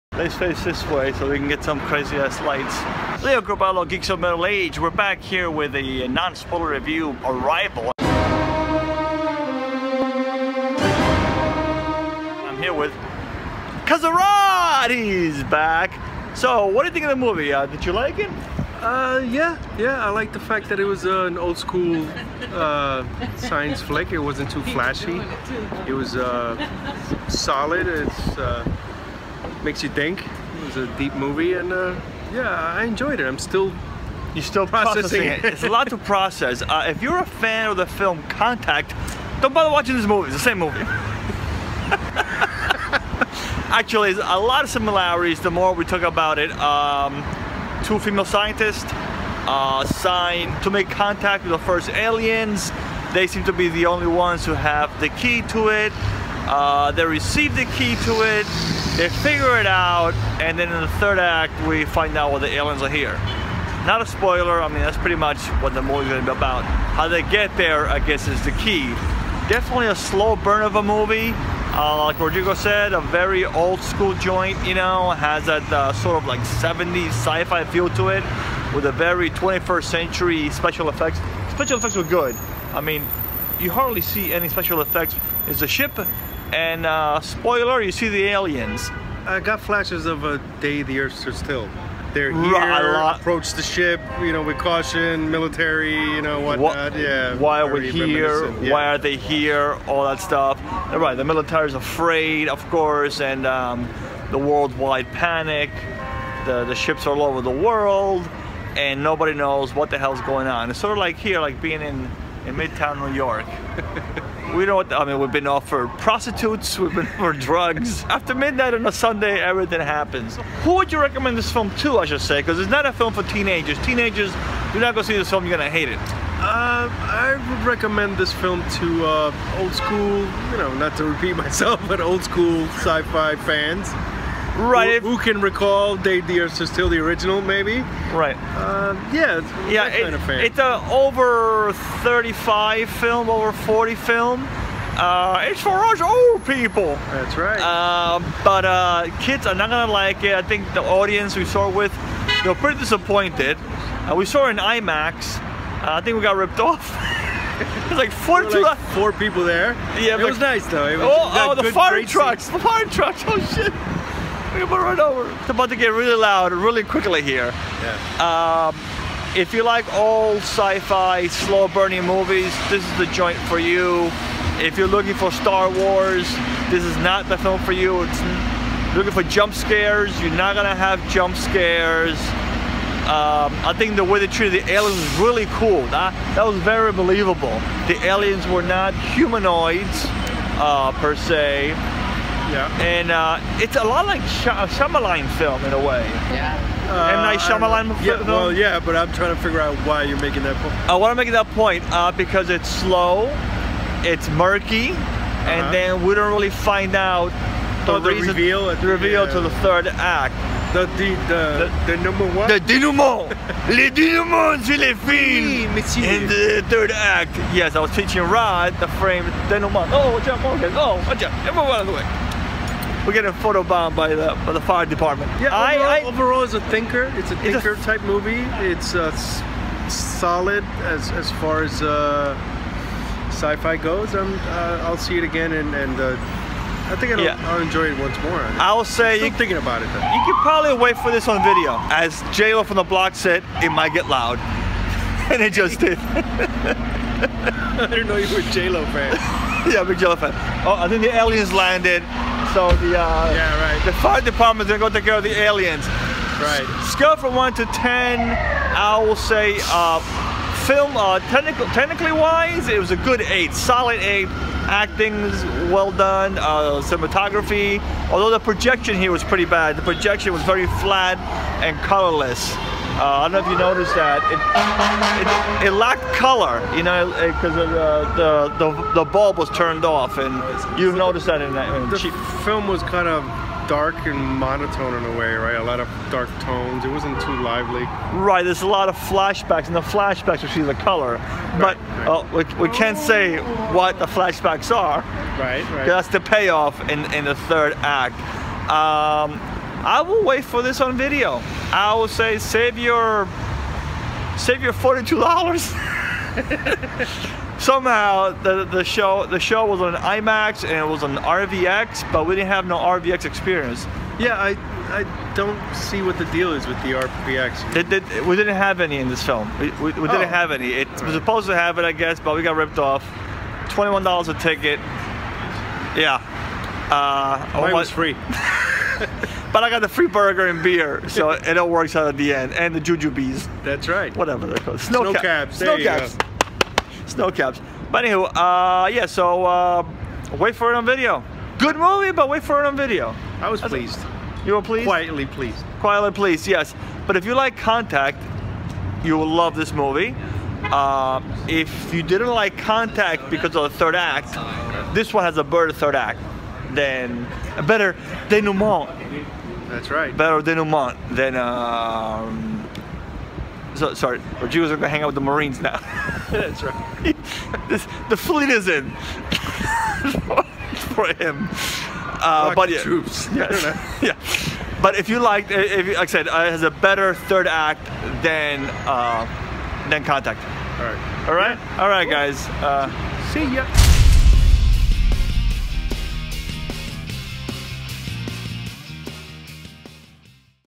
Arrival Non Spoiler Review
We review the science fiction film ARRIVAL minutes after leaving theater on 42bd Street in Manhattan. Directed by Denis Villeneuve and starring Amy Adams, Jeremy Renner, and Forest Whittaker.